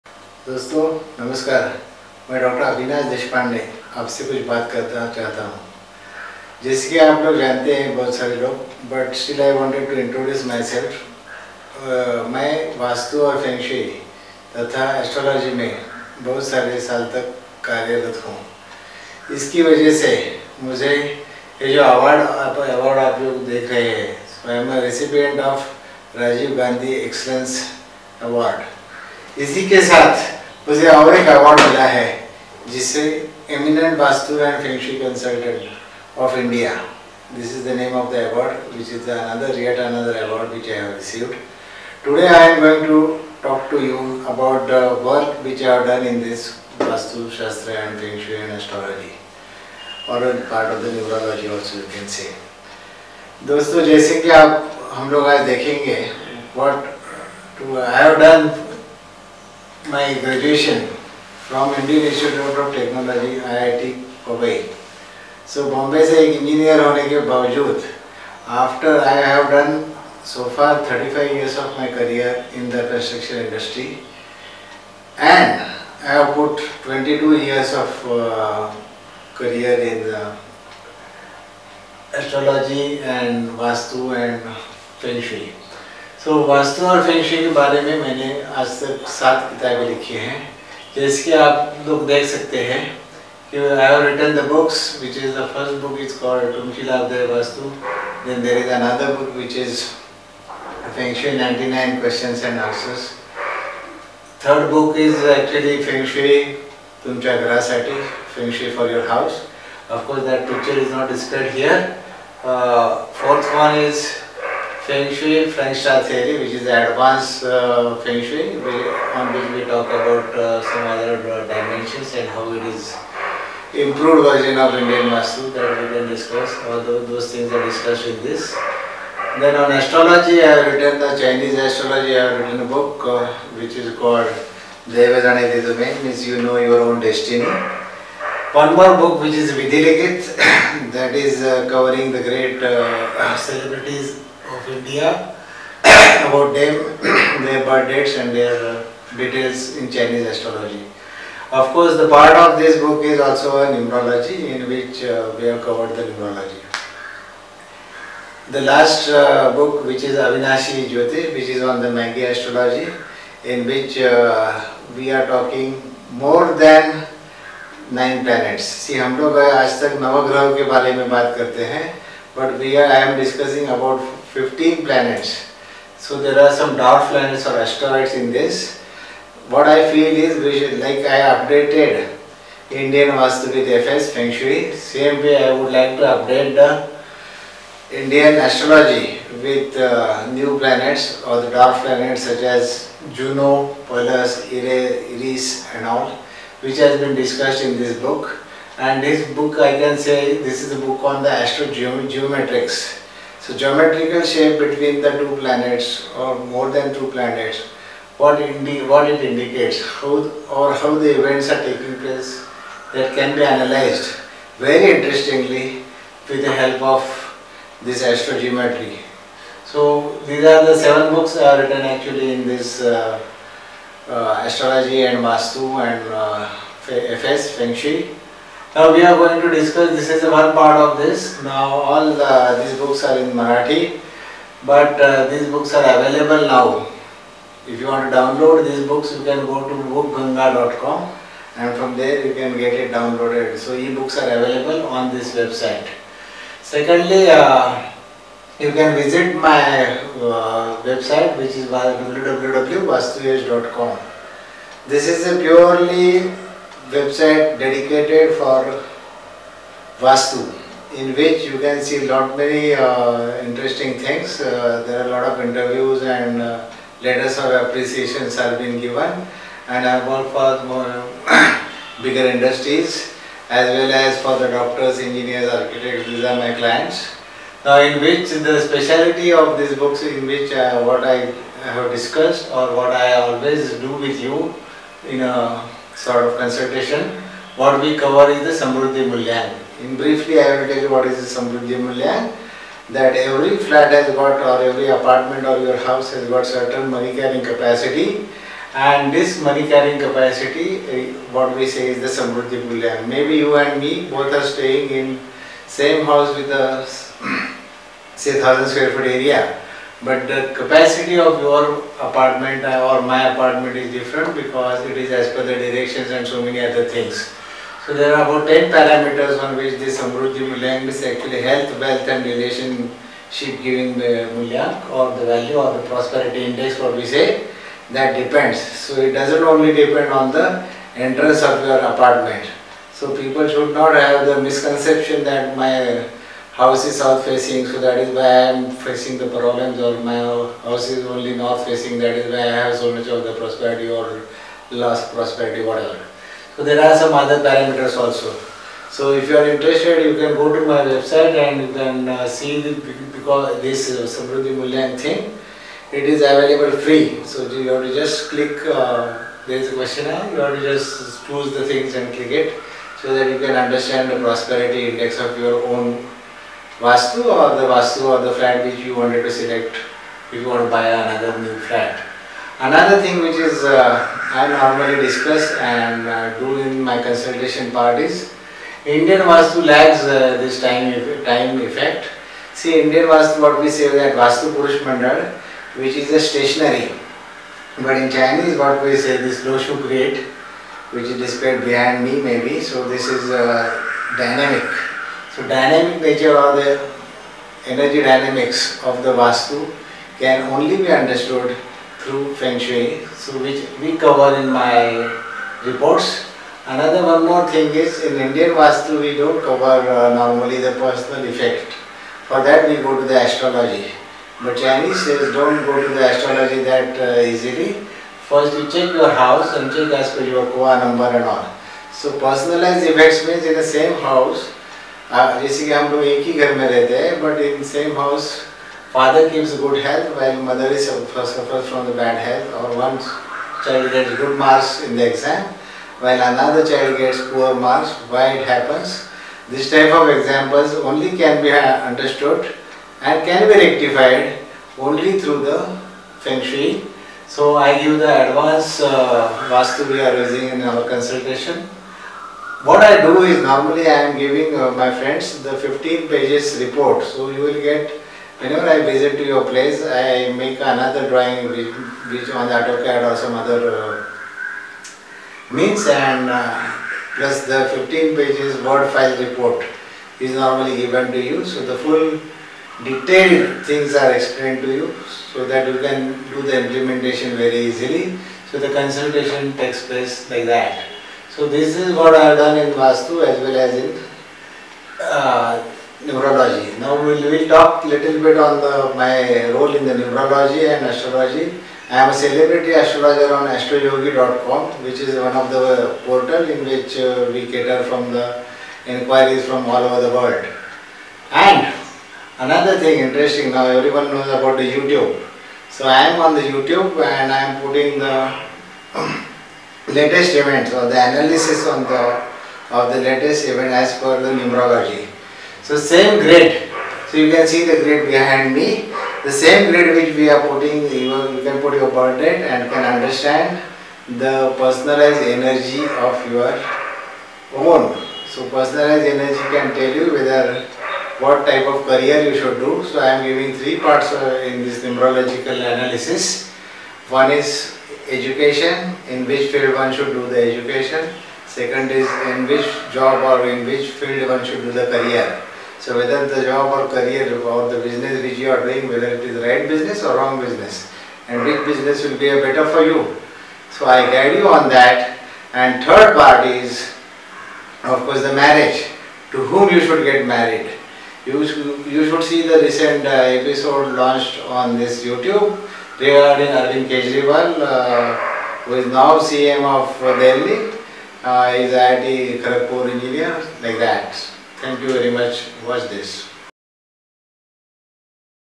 Vastuyash - Dr.Avinash Deshpande - Why we are # 1 in Vaastu & astrology
Dr.Avinash Deshpande - IIT Mumbai Engineer. Actual a non believer in Vaastu & astrology came into this to disprove these sciences !! He have 22 years of experience in Vaastu & astrology + 32 years of sound experience in all branches of construction engg. Worked in India & abroad. Please visit our web site vastuyash.